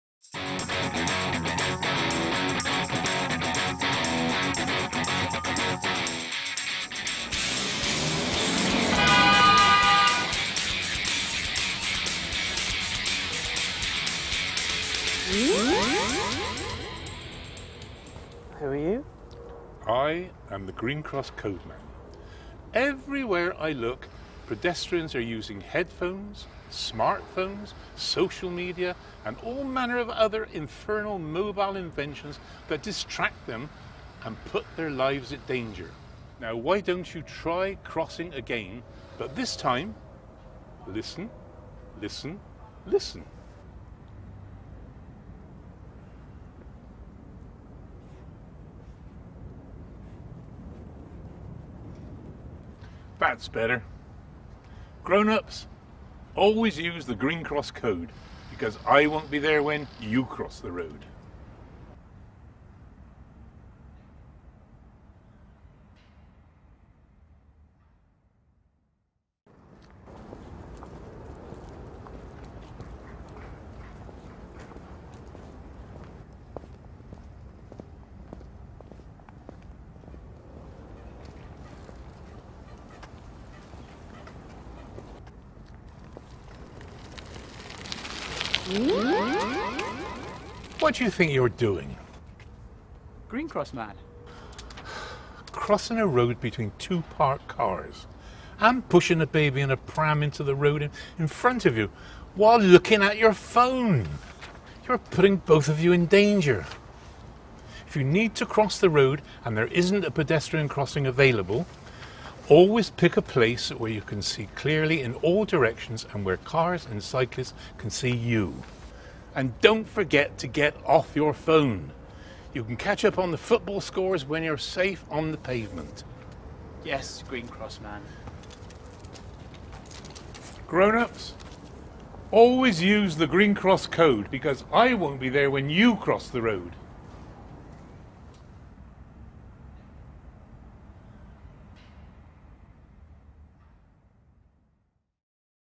David Prowse RETURNS as The Green Cross...
For Road Safety Week 2014, and after a 40 year-hiatus, The Green Cross Code Man, David Prowse, has returned - only this time teaching todayÂ¿s careless headphone-wearing, smartphone-addicted adults a thing or two about the Green Cross Code.